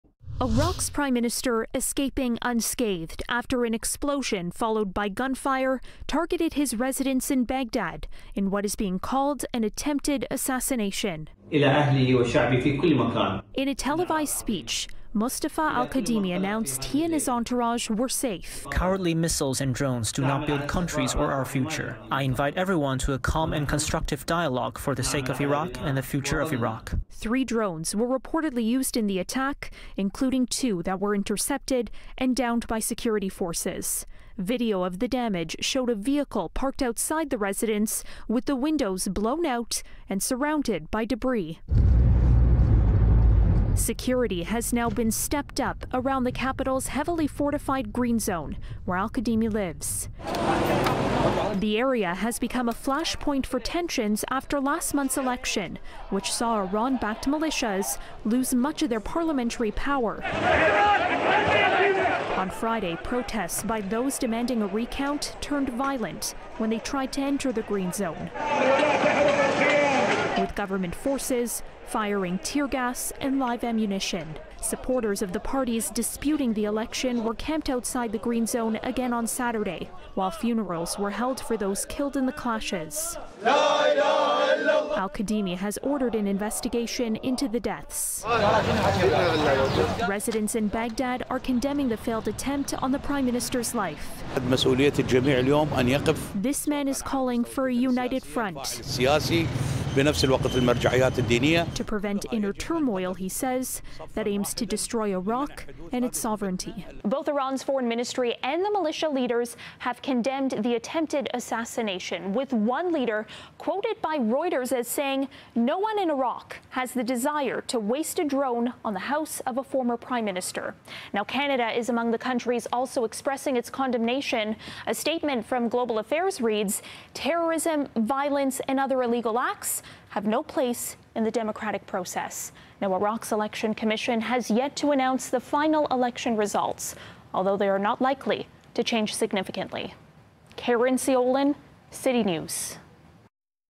Iraq’s prime minister survives assassination attempt
Iraq’s prime minister survived a drone strike on his home in what Iraqi officials are calling an attempted assassination. Caryn Ceolin with why the attack is raising tensions over recent parliamentary election results.